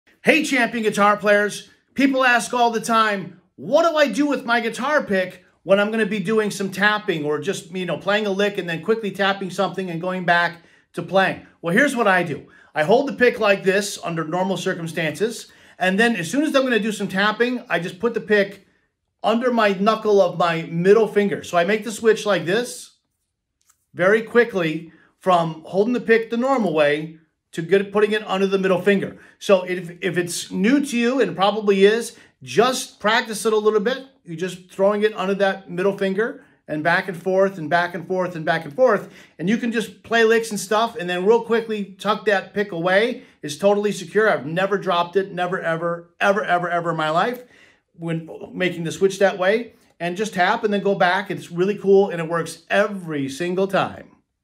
How To Hold The Pick When Tapping
People ask me all the time: What do I do with my guitar pick when I'm going to do some tapping? Here is what I do.